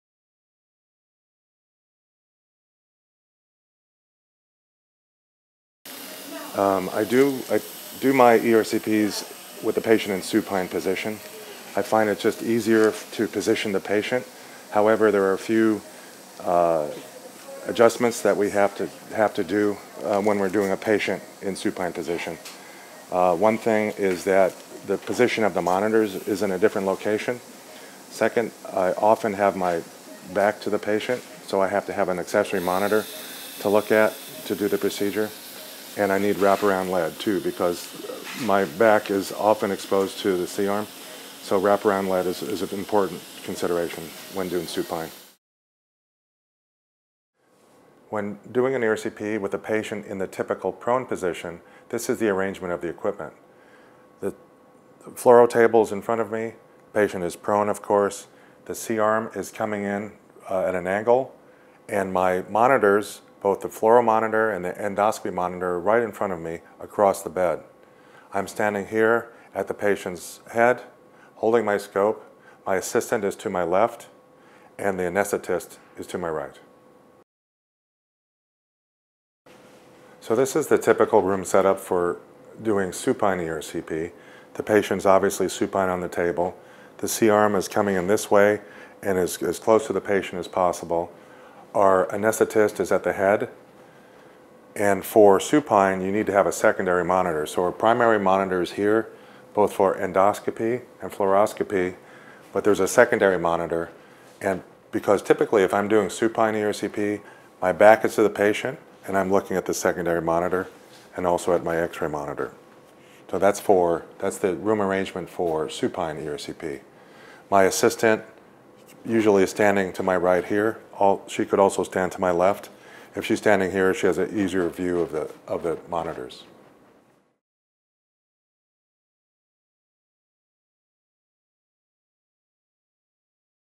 ERCP Patient Positioning
Dr. David Diehl discusses patient positioning considerations for ERCP procedures.

Dr. David Diehl was a paid consultant for Cook Medical at the time this video was made.